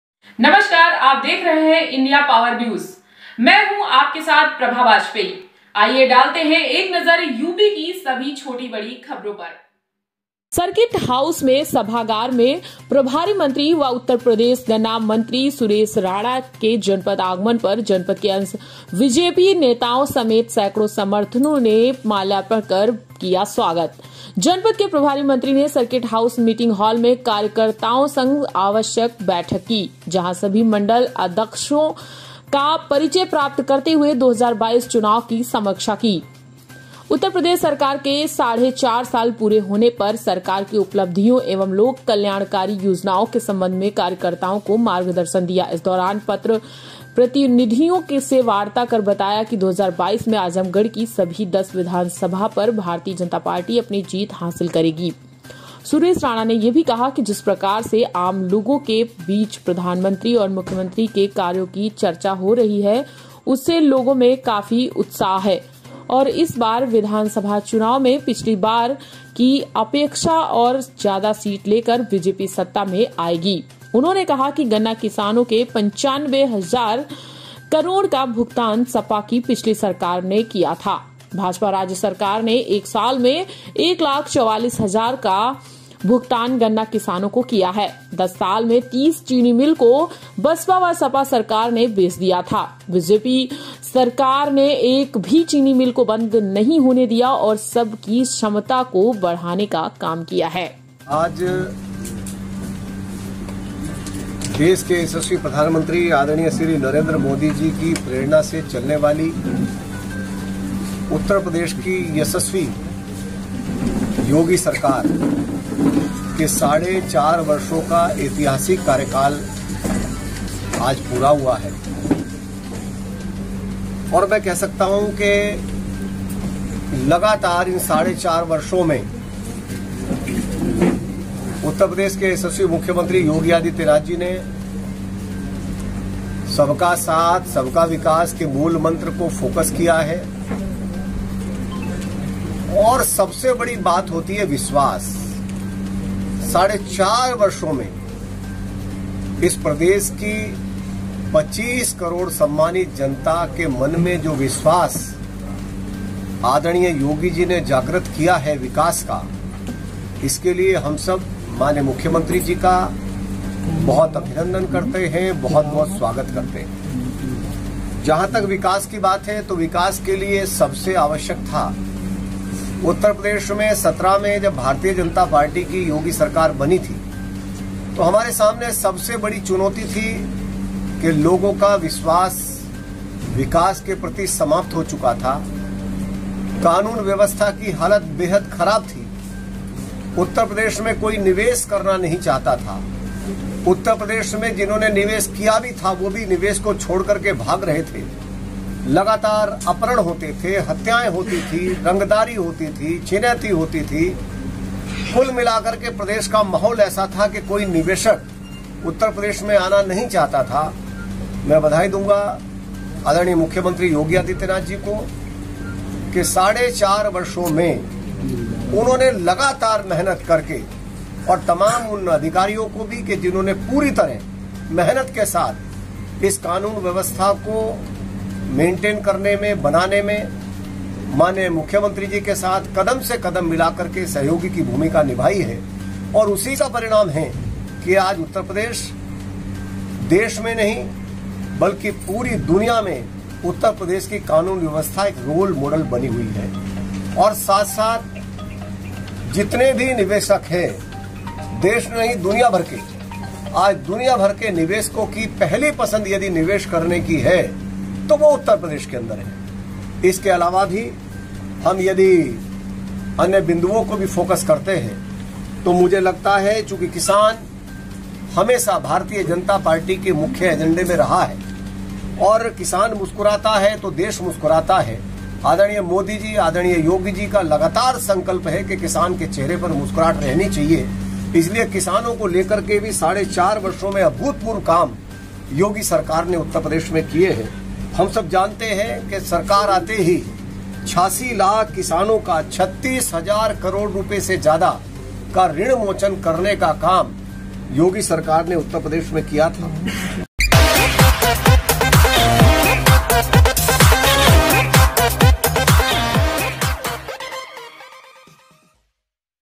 UP:पिछली बार से ज्यादा मिलेगी सीटे सड़क,बिजली,फ्री राशन,पेंशन उपलब्धि आदि-सुरेश राणा
आजमगढ़ : सर्किट हाउस में सभागार में  प्रभारी मंत्री व उ0प्र0 गन्ना मंत्री सुरेश राणा के जनपद आगमन पर जनपद की अंश बीजेपी नेताओं समेत सैकड़ों समर्थकों ने माल्यार्पण कर किया स्वागत किया। जनपद के प्रभारी मंत्री ने सर्किट हाउस मीटिंग हॉल में कार्यकर्ताओं संग आवश्यक बैठक की। जहां सभी मंडल अध्यक्षों का परिचय प्राप्त करते हुए 2022 चुनाव की समीक्षा की। उत्तर प्रदेश सरकार के साढ़े 4.5 वर्ष पूरे होने पर सरकार की उपलब्धियों एवं लोक कल्याणकारी योजनाओं के संबंध में कार्यकर्ताओं को मार्गदर्शन दिया। इस दौरान पत्र प्रतिनिधियों से वार्ता कर बताया कि 2022 में आजमगढ़ की सभी 10 विधानसभा पर भारतीय जनता पार्टी अपनी जीत हासिल करेगी। सुरेश राणा ने कहा कि जिस प्रकार से आम लोगों के बीच प्रधानमंत्री और मुख्यमंत्री के कार्यों की चर्चा हो रही है। उससे लोगों में काफी उत्साह है और इस बार विधानसभा चुनाव में पिछली बार की अपेक्षा और ज्यादा सीट लेकर बीजेपी सत्ता में आएगी। उन्होंने कहा कि गन्ना किसानों के 95 हजार करोड़ का भुगतान सपा की पिछली सरकार ने किया था। भाजपा राज्य सरकार ने 1 साल में ₹1 लाख 44 हजार का भुगतान गन्ना किसानों को किया है। 10 साल में 30 चीनी मिल को बसपा व सपा सरकार ने बेच दिया था। बीजेपी सरकार ने एक भी चीनी मिल को बंद होने नहीं दिया और सब की क्षमता को बढ़ाने का काम किया है।